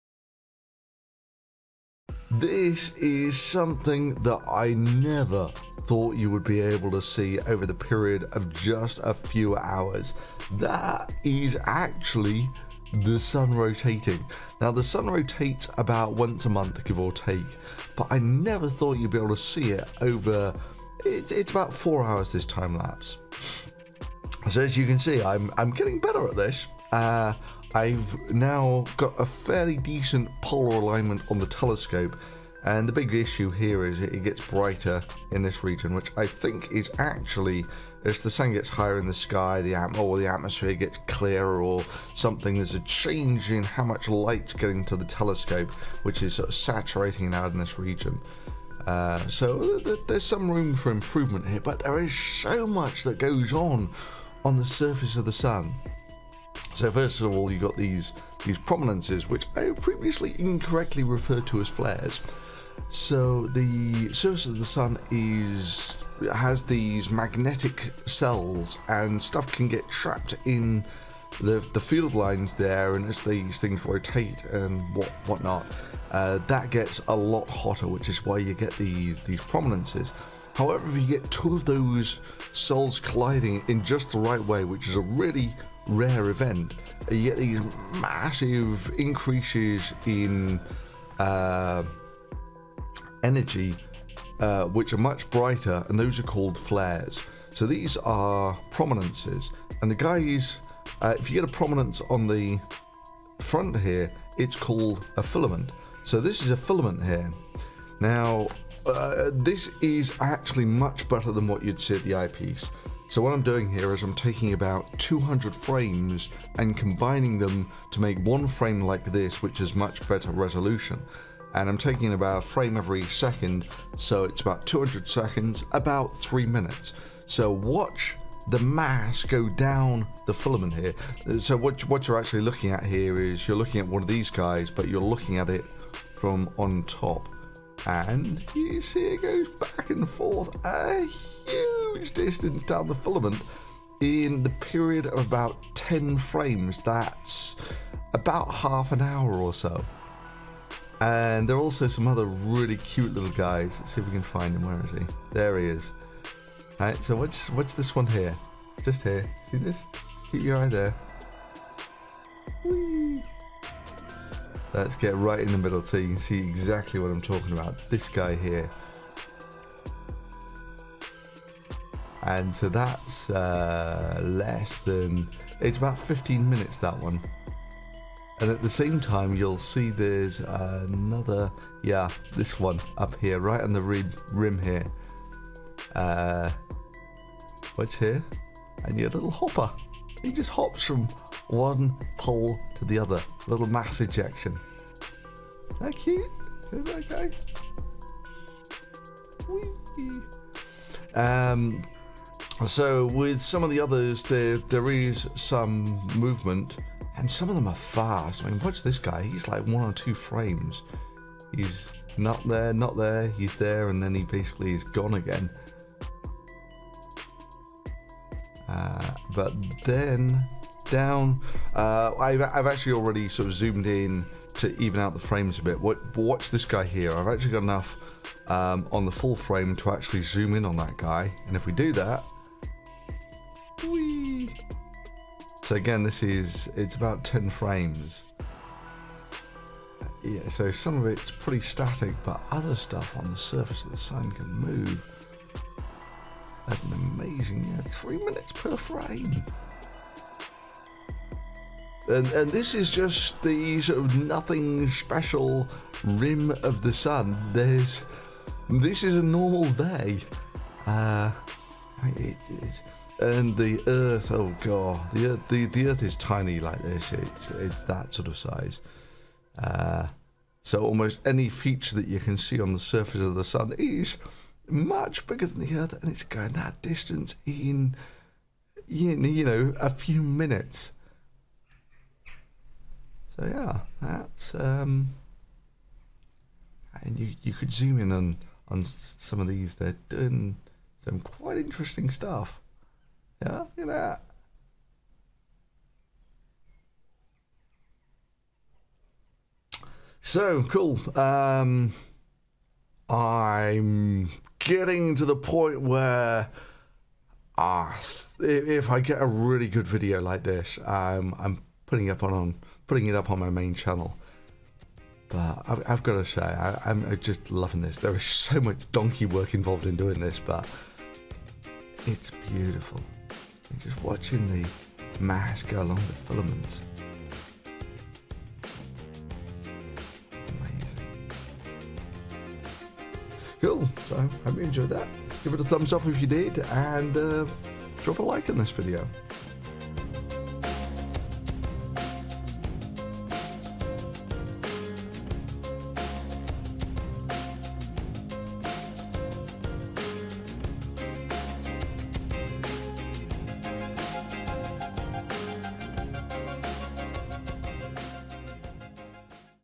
I CAN SEE THE SUN ROTATING!!!!
This is something I never thought you would be able to see over just a few hours. then while processing the timelipse... JAW MEET FLOOR!

the telescope here is a double stacked lunt 80.  A very expensive telescope.

mount is a german equatorial skywatcher

The camera is a skyris 236C.  I got it specially for this and have to admit it preforms very well!